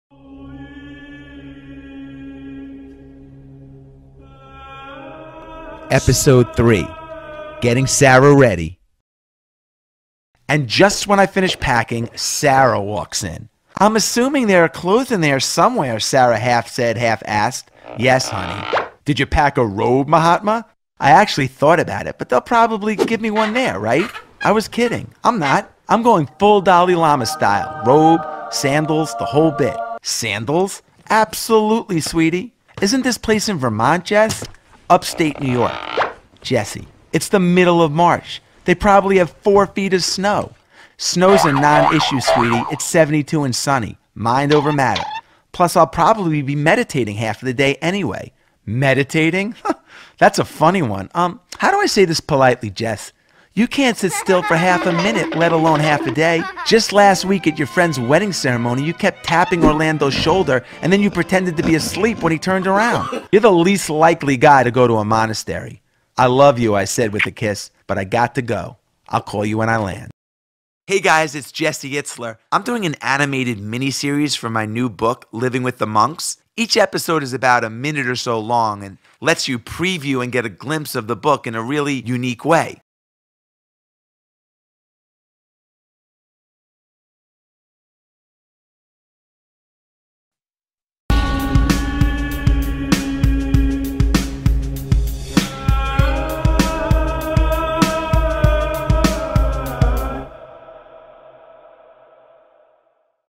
Living with the Monks | Episode 3: Getting Sara Ready
The animated mini-series based on my experience living with the monks.  What turning off my phone taught me about happiness, gratitude and focus.

Episode 3: Getting Sara Ready